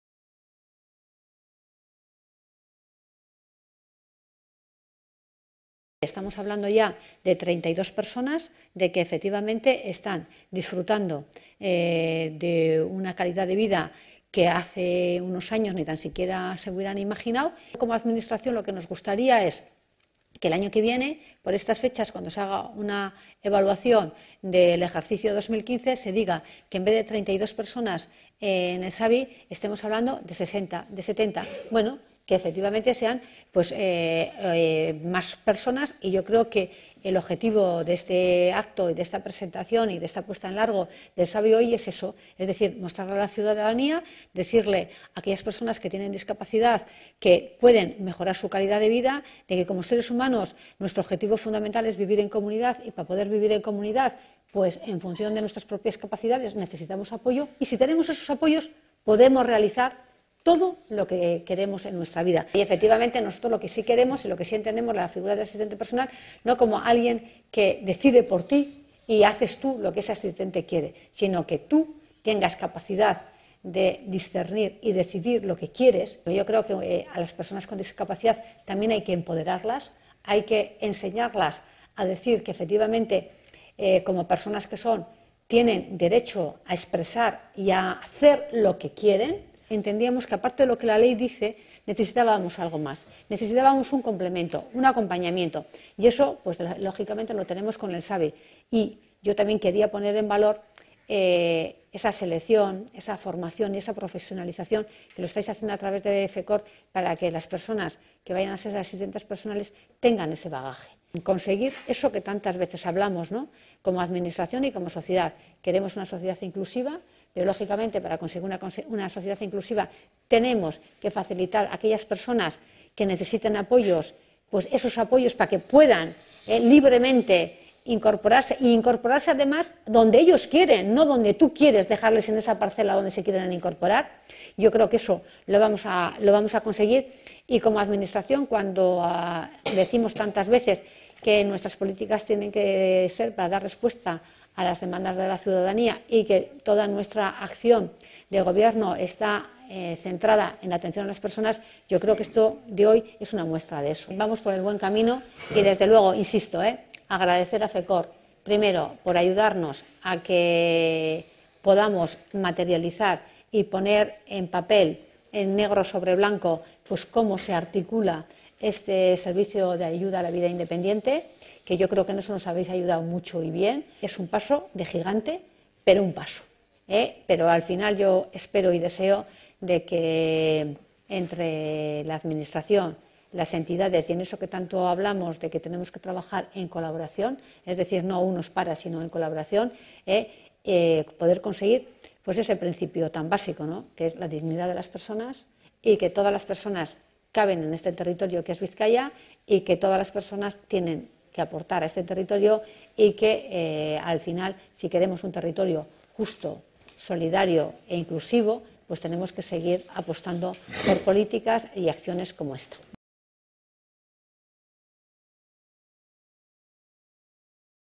Independente bizitzen laguntzeko zerbitzu berria / Nuevo servicio de apoyo a la vida independiente
Bizkaiko Foru Aldundiak laguntza pertsonala emateko eta independente bizitzen laguntzeko zerbitzu berri bat jarri du abian, Fekoor-ek kudeatua. Zerbitzua beste aurrerapauso bat da autonomia pertsonalaren eta partaidetza komunitarioaren sustapena bermatzeko laguntza-beharrizanen estalduran.
La Diputación Foral de Bizkaia pone en marcha un nuevo servicio de asistencia personal y apoyo a la vida independiente gestionado por Fekoor. Supone un nuevo paso adelante en la cobertura a las necesidades de apoyo para garantizar el fomento de la autonomía personal y la participación comunitaria.